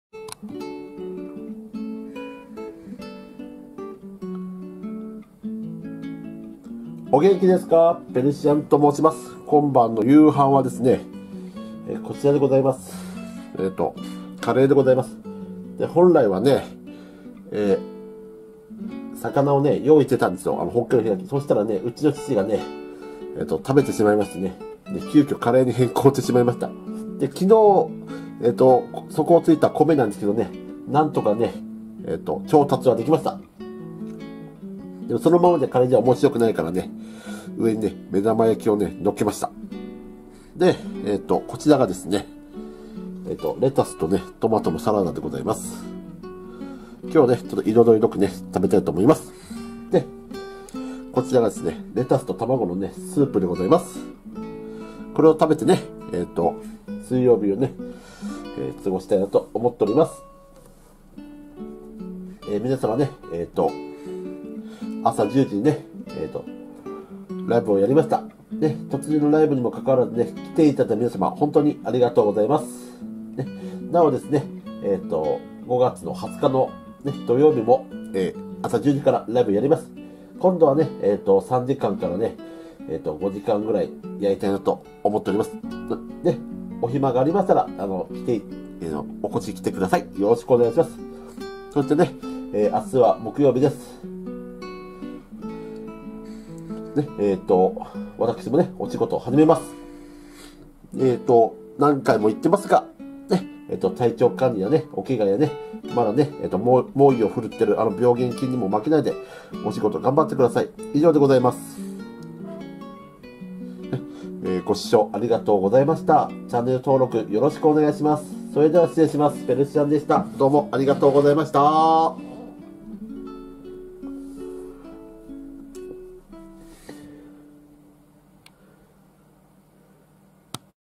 ５月５日の夕飯はカレー丼とサラダとレタススープです！✨
本来はホッケの開きにしようと思ったのですが！父が食べてしまったので、急遽カレー丼にしました✨後はレタスとトマトのサラダに卵スープです！
載せてはいないのですが！酒はジムビームのスプライト割です✨

【今度の予定】※変更有
５/８　YouTubeライブ
５/15    YouTubeライブ（13時から）
５/20    YouTubeライブ
５/27　YouTubeライブ